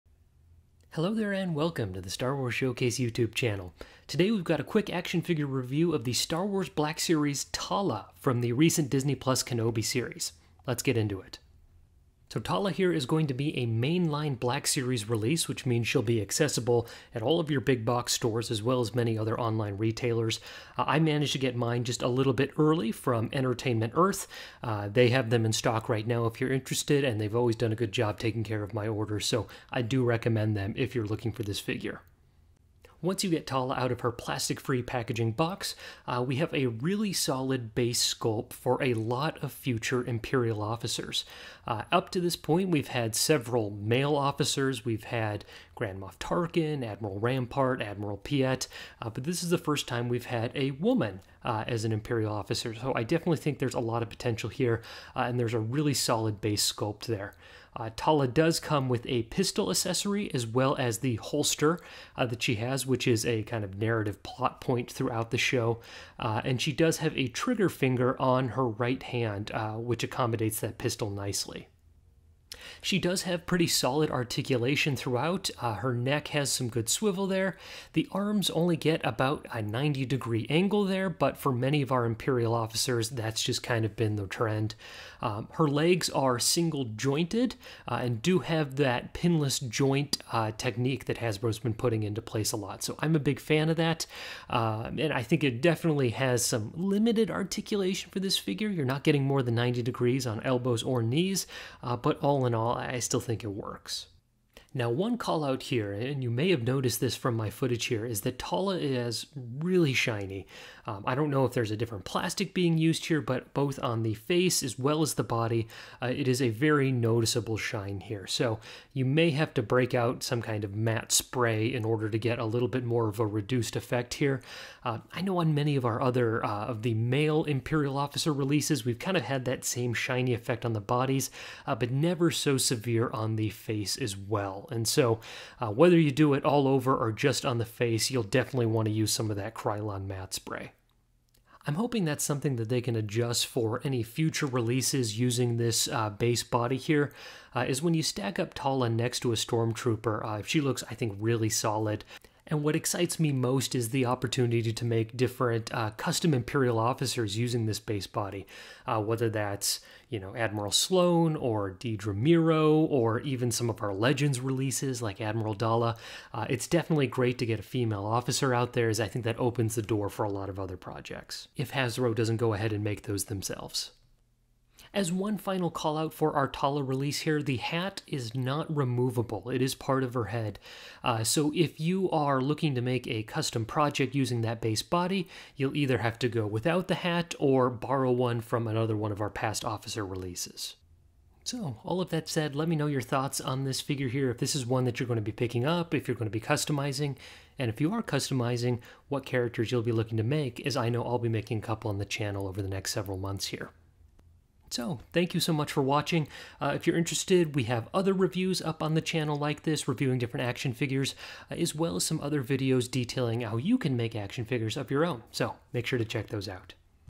Star Wars Black Series TALA (IMPERIAL OFFICER) | Action Figure Review
Let's take a look at Tala, the Imperial turncoat whose figure promises a lot more Imperial Officers in the future!